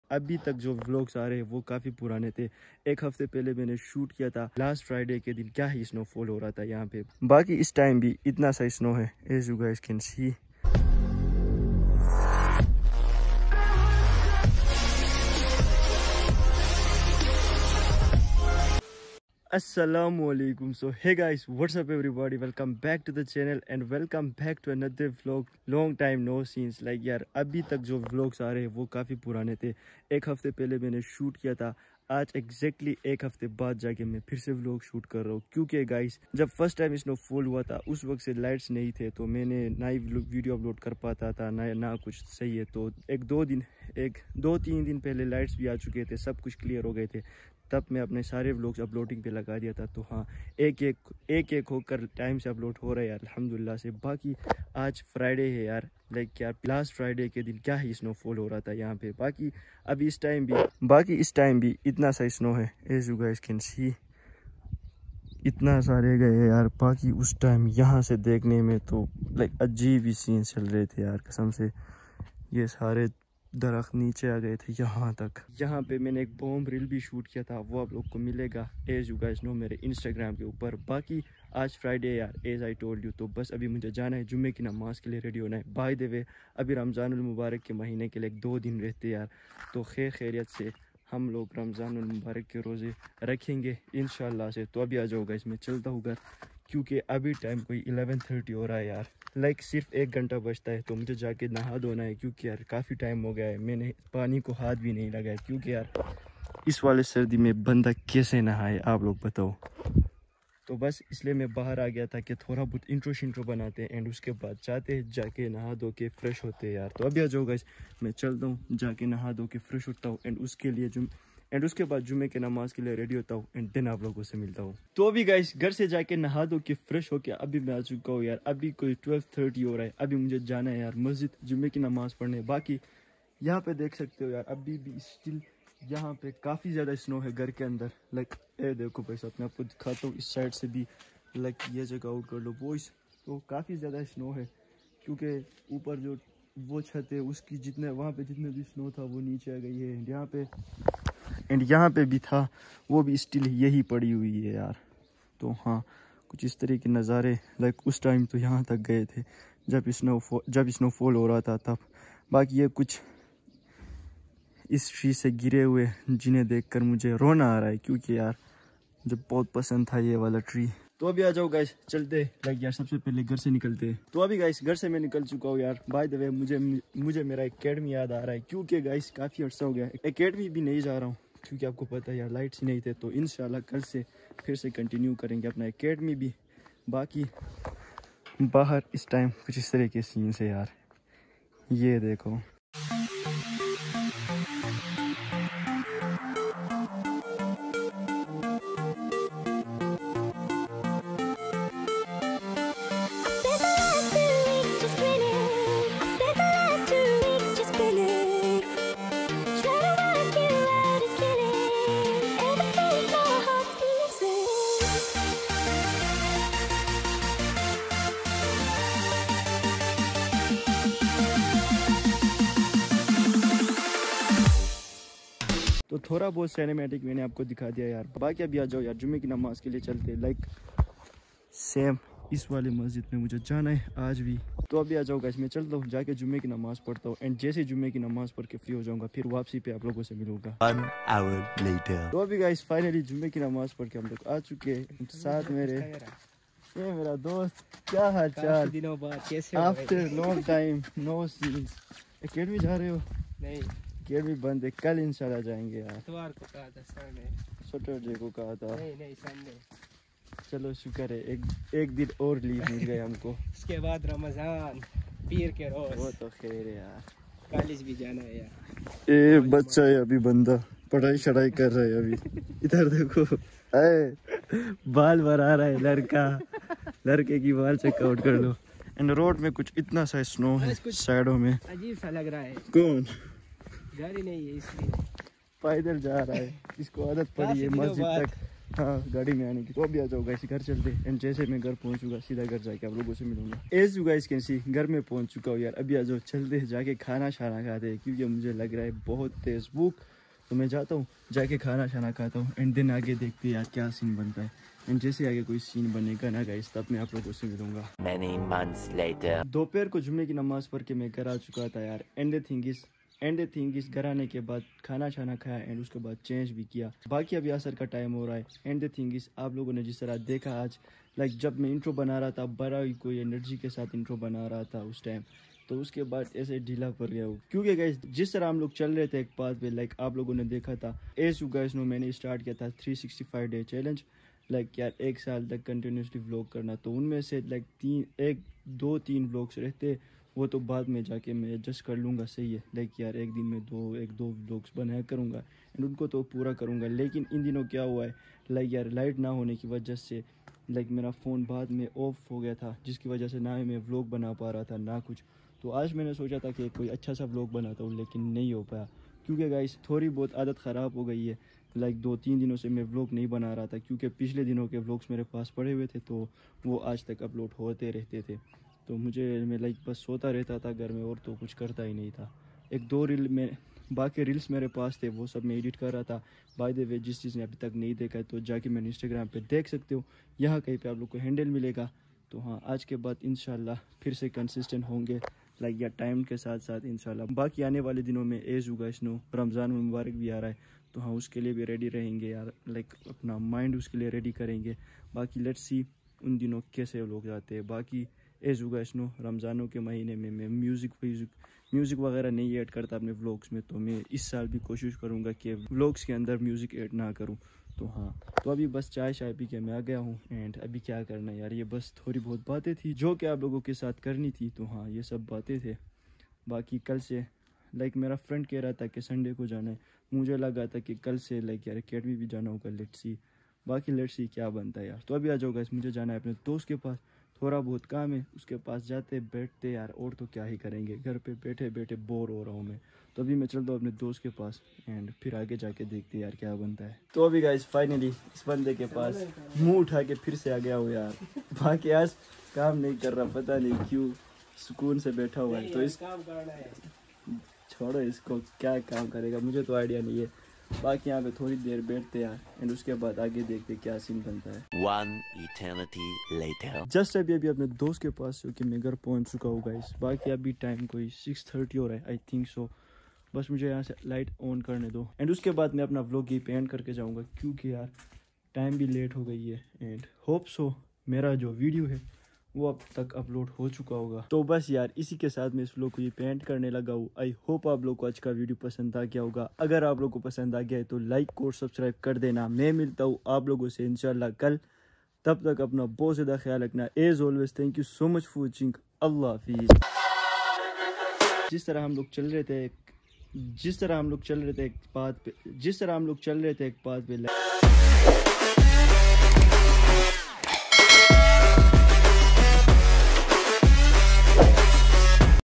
Snowfall Ki 7 Dinon Ke Bad Bhi Itna Zyada Snow Hai Mere Gaon Mein 😳 | BILAL RUMI
Do like and drop your valuable comments